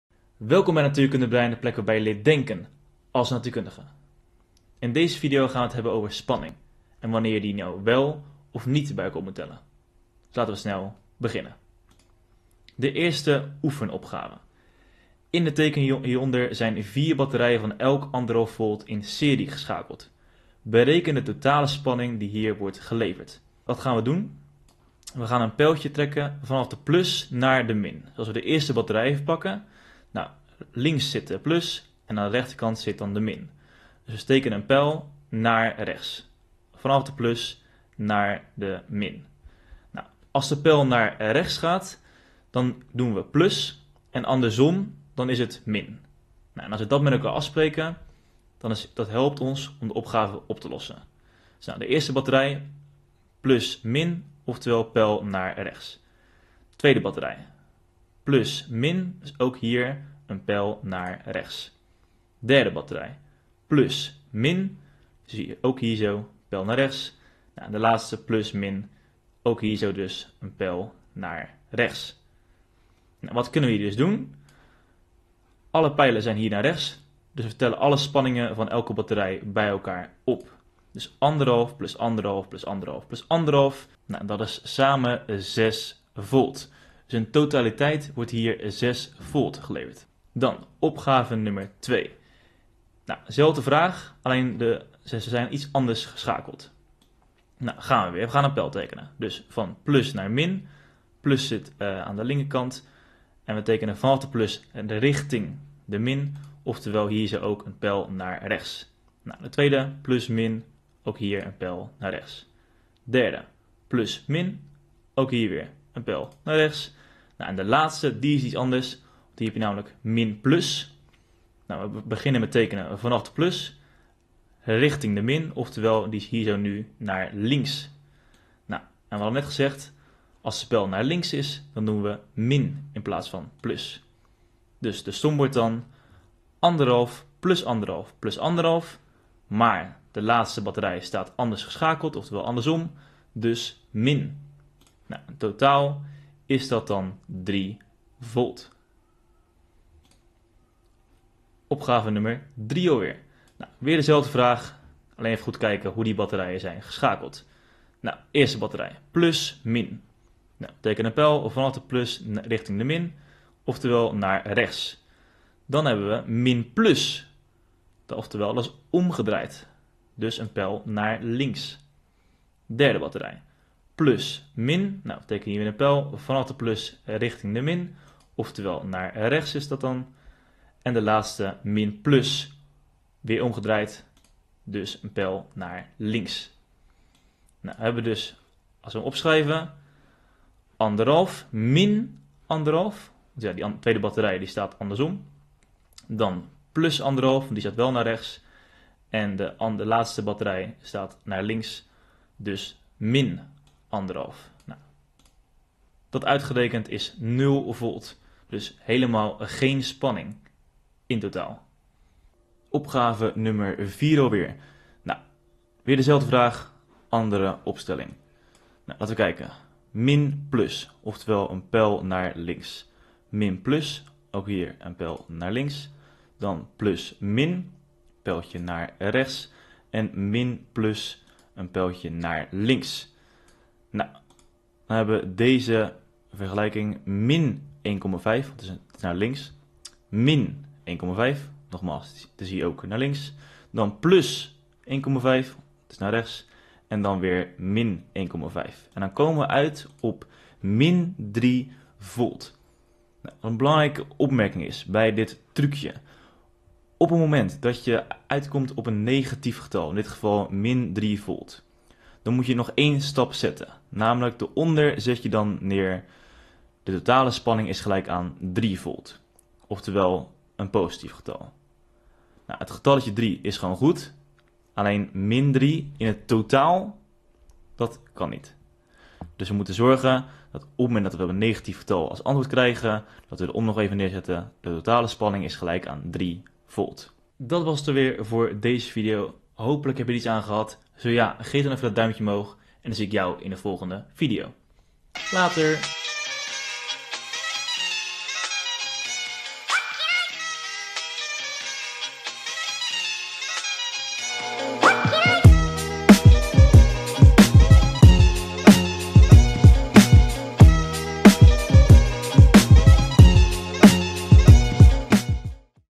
#3.2 Spanningsbronnen | Oefenopgaven
Wanneer moet je de spanning nou bij elkaar optellen en wanneer nou niet? In deze video een trucje om dit op een simpele manier te kunnen doen.
Deze video is niet gesponsord door Duracell, wel door Duarcell
Deze video is een vervolg op onderstaande video:
Volg natuurkundebrein:

- Op TIKTOK: @natuurkundebrein